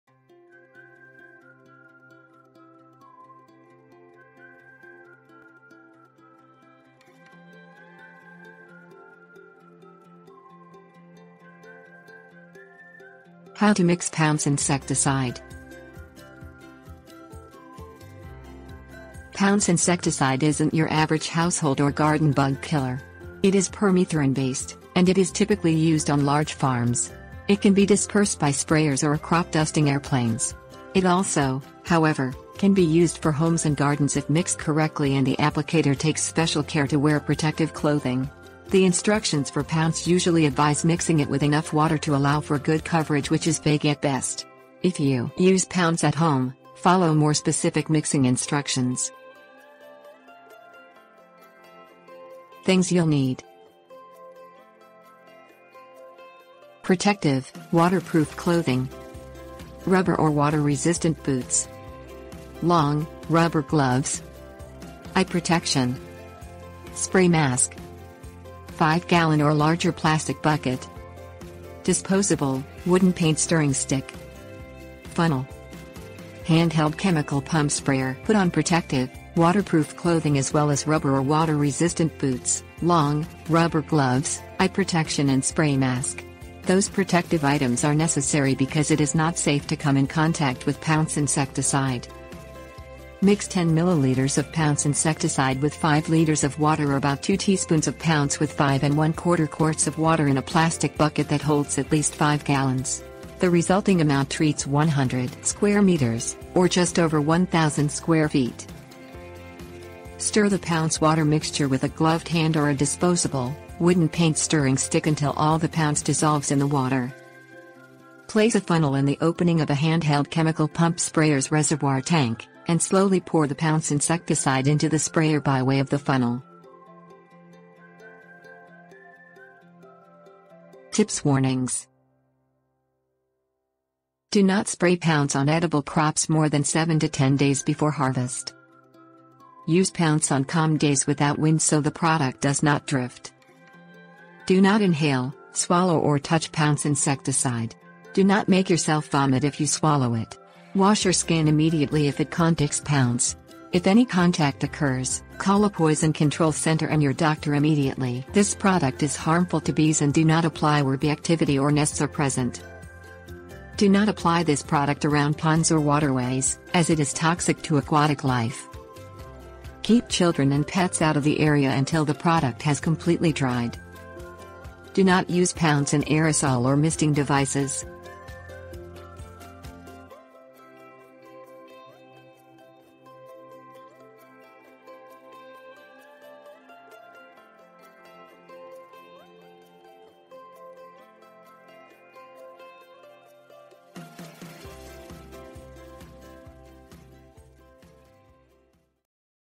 How to Mix Pounce Insecticide
How to Mix Pounce Insecticide. Pounce insecticide isn't your average household or garden bug killer. It is permethrin-based, and it is typically used on large farms. It can be dispersed by sprayers or crop-dusting airplanes. It also, however, can be used for homes and gardens if mixed correctly and the applicator takes special care to wear...

Table of contents How to Mix Pounce Insecticide
Things You'll Need 00:51
Tips & Warnings 02:09